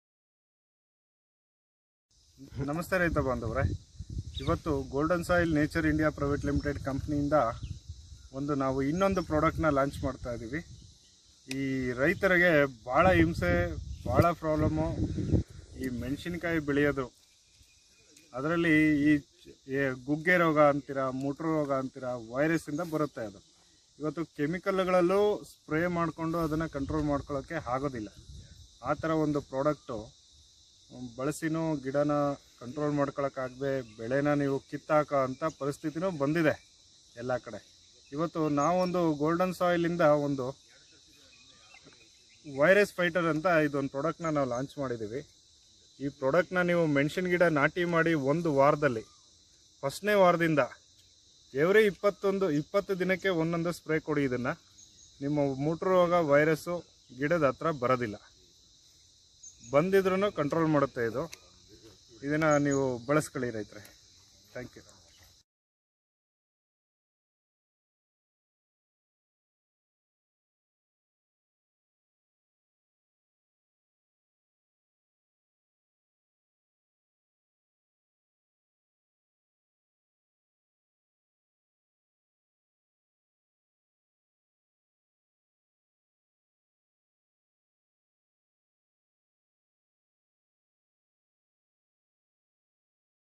ಮೆಣಸಿನಕಾಯಿ ಮುಟುರು ರೋಗಕ್ಕೆ ಪರಿಣಾಮಕಾರಿ ಔಷಧಿ ಗೋಲ್ಡನ್ ಸಾಯಿಲ್ ವೈರಸ್ ಫೈಟರ್
ಮುಟುರು ರೋಗ ಸಾಮಾನ್ಯವಾಗಿ ಮೆಣಸಿನಕಾಯಿ ತೋಟದಲ್ಲಿ ಕಂಡುಬಂದು ರೈತರಿಗೆ ಅತಿಯಾದ ನಷ್ಟವನ್ನು ಉಂಟು ಮಾಡುತ್ತದೆ ಯಾವುದೇ ಔಷಧದಿಂದಲೂ ಕೂಡ ಸುಲಭವಾಗಿ ನಿವಾರಣೆಯಾಗುವುದಿಲ್ಲ ಗೋಲ್ಡನ್ ಸಾಯಿಲ್ ವೈರಸ್ ಫೈಟರ್ ಪ್ರಾರಂಭಿಕ ಹಂತದಿಂದಲೇ ಬಳಸುವುದರಿಂದ ಈ ರೋಗವನ್ನು ತಡೆಯಬಹುದು ಅಥವಾ ರೋಗ ಕಾಣಿಸಿಕೊಂಡ ಪ್ರಥಮ ಹಂತದಿಂದಲೇ ಉಪಯೋಗಿಸುವುದರಿಂದ ರೋಗದಿಂದ ಹಾನಿ ಉಂಟಾಗುವುದನ್ನು ತಪ್ಪಿಸಬಹುದು ಆದ್ದರಿಂದ ಮೆಣಸಿನಕಾಯಿ ಮುಟುರು ರೋಗಕ್ಕೆ ಪರಿಣಾಮಕಾರಿ ಔಷಧಿ ಗೋಲ್ಡನ್ ಸಾಯಿಲ್ ವೈರಸ್ ಫೈಟರ್